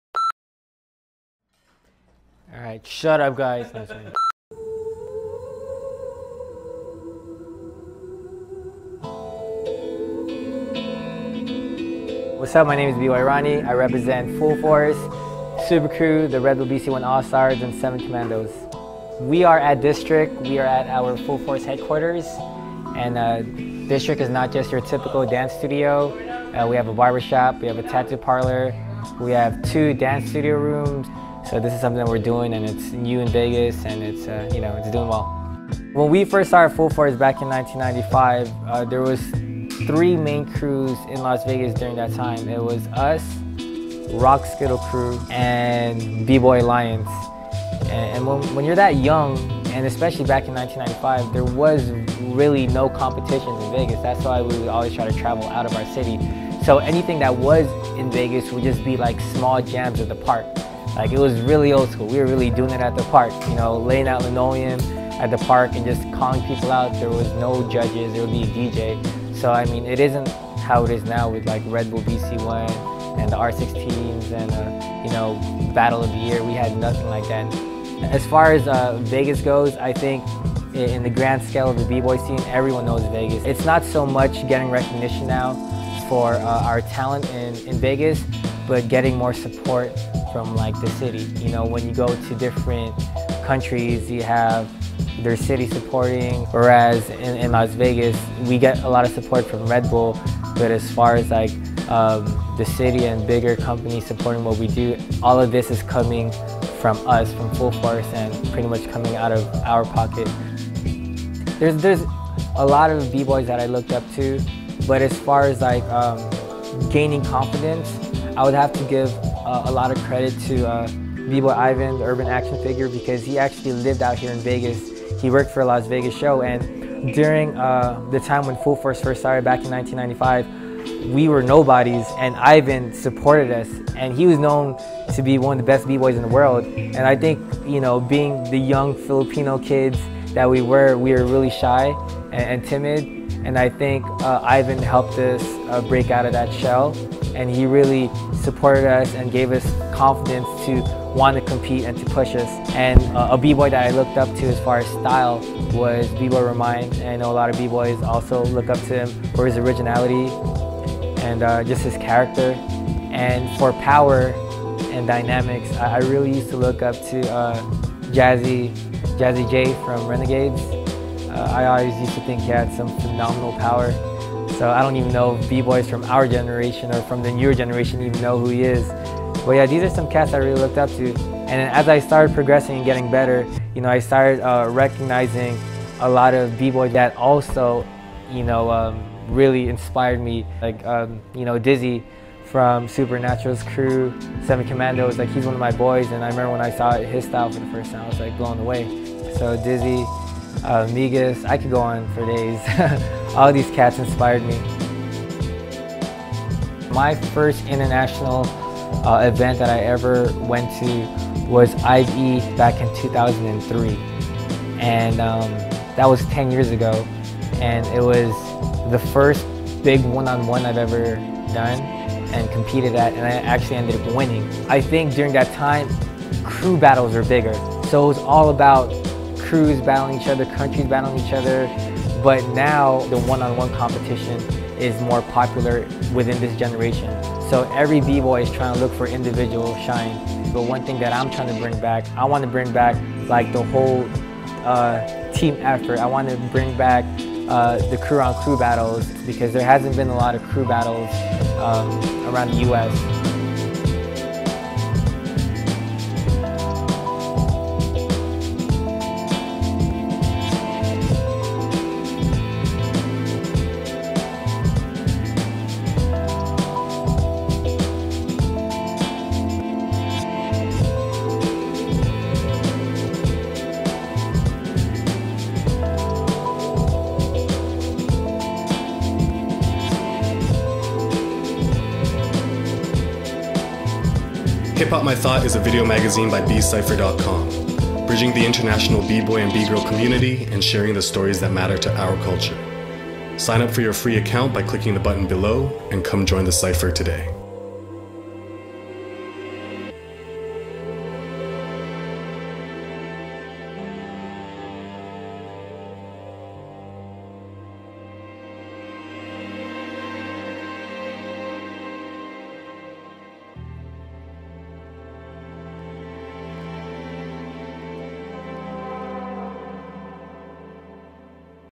HIP HOP: MY THOUGHT // EP.5 // BBOY RONNIE // BCYPHER.COM
Episode 5 of Hip Hop: My Thought ft. BBoy Ronnie
Awesome interview condensed here to just 5 minutes; so if you want to hear the full raw and uncut interview, and check out other behind the scenes awesomeness, head over to the RONNIE BEHIND THE SCENES

Filmed and Edited by: TW @MauriceChang // IG @Maurice_Chang
Soundtrack Produced by: Danny "deeB" Van Den Hoek